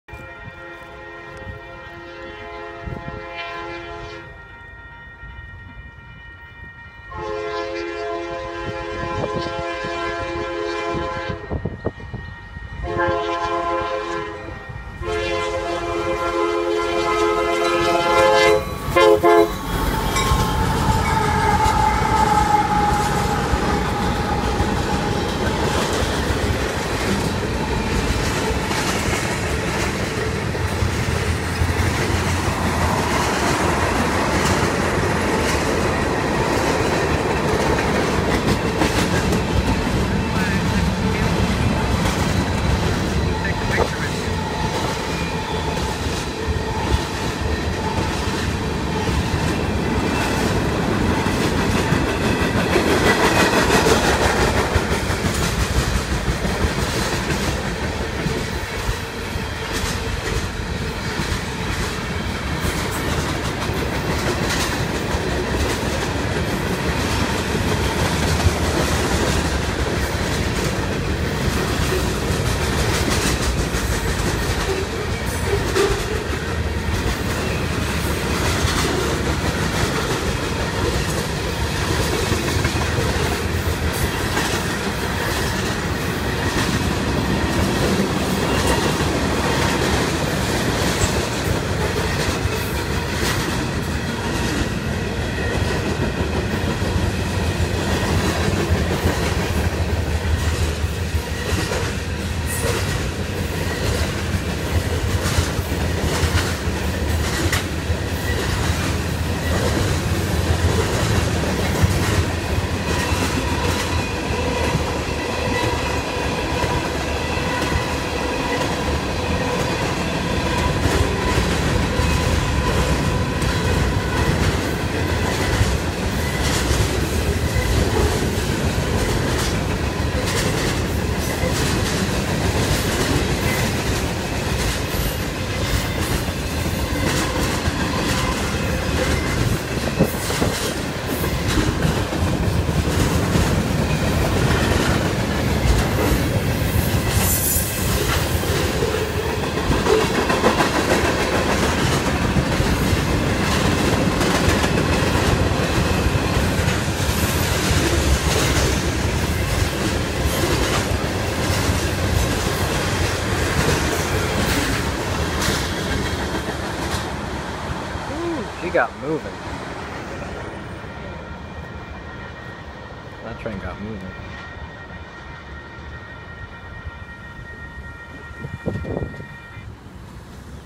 A UP Manifest Races out of Fort Worth, TX! | Saginaw Shorts
In the Season Finale Of TRAINS OF we'll be airing a week long special dedicated to all the traffic we saw in our trip to 24 Hours at Saginaw! There are 11 brand new episodes to close out season 2! So enjoy and be watching for tomorrows episode!